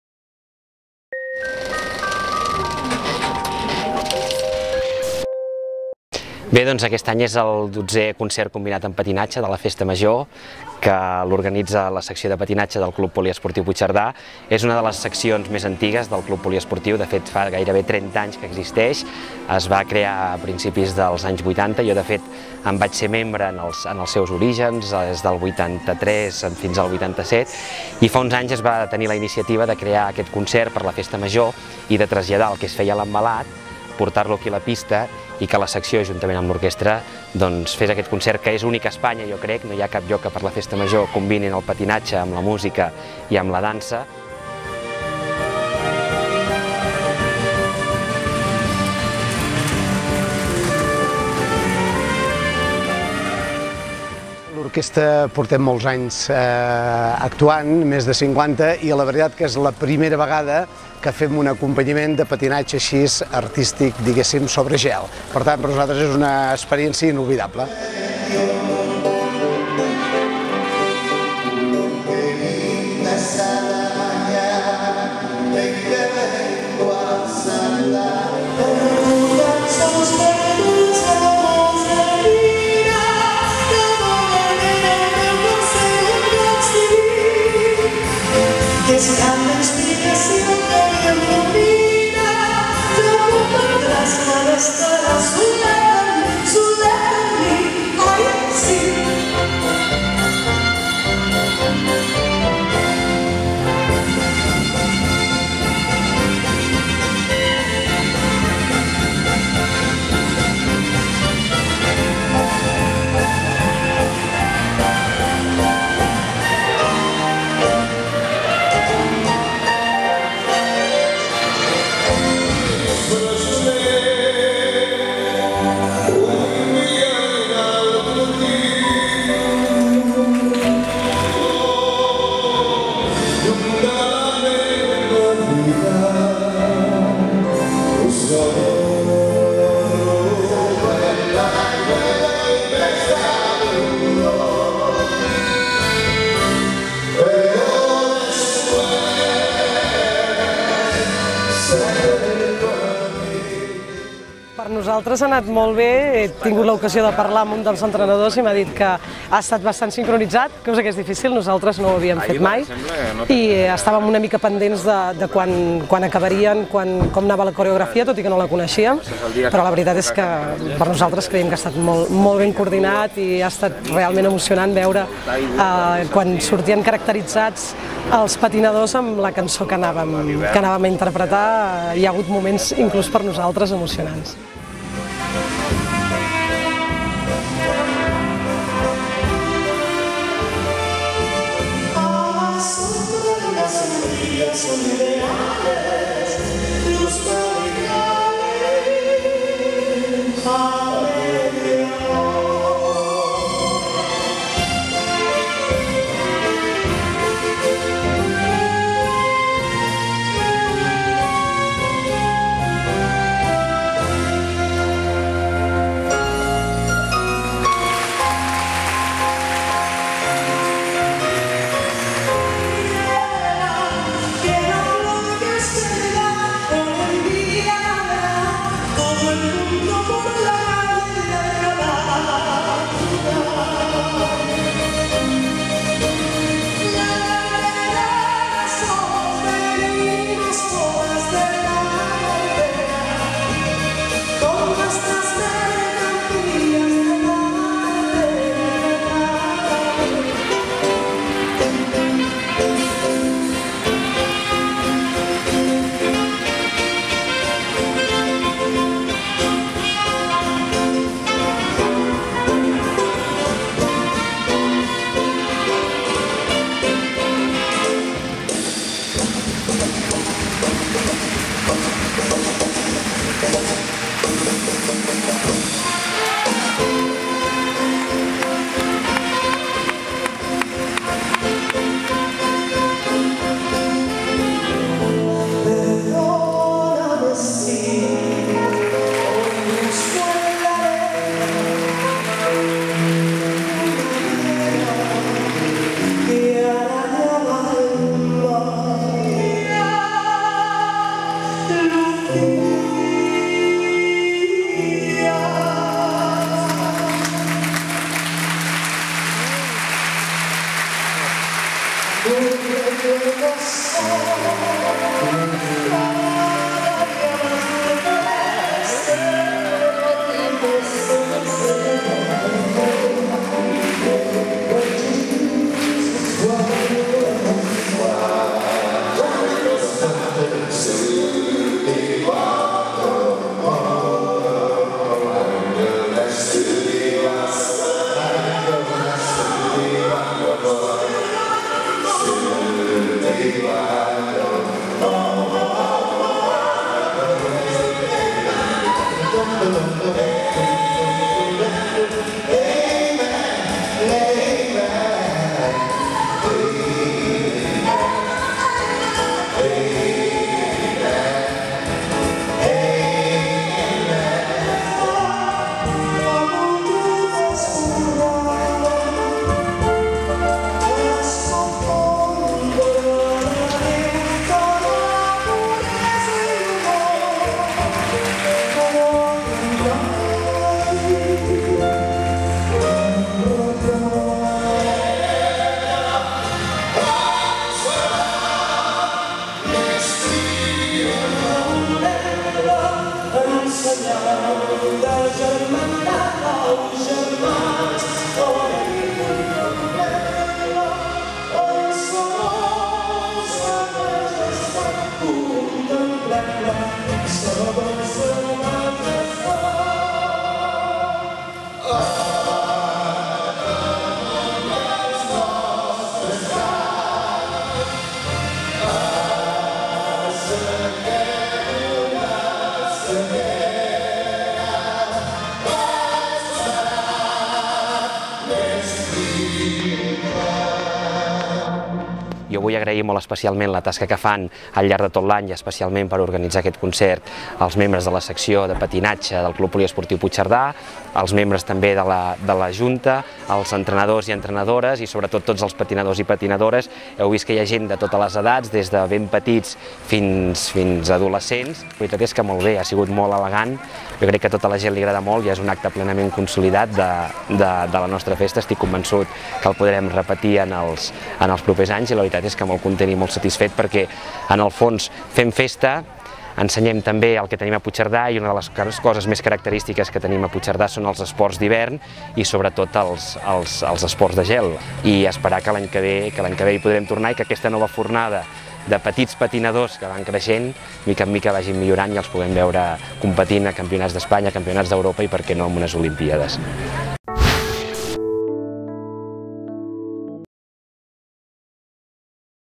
2011-07-17 XVII CONCERT COMBINAT DE PATINATGE ARTÍSTIC - MEMORIAL RAMON CONDOMINES
XVII CONCERT COMBINAT DE PATINATGE ARTÍSTIC - MEMORIAL RAMON CONDOMINES

Per les Festes del Roser, la secció de patinatge del Club Poliesportiu Puigcerdà, organitza la XVII edició del Memorial Ramon Condomines, antic fundador del club.
Enguany, l'Orquestra Setson ha acompanyat als patinadors en una combinació de música i patinatge única a Espanya.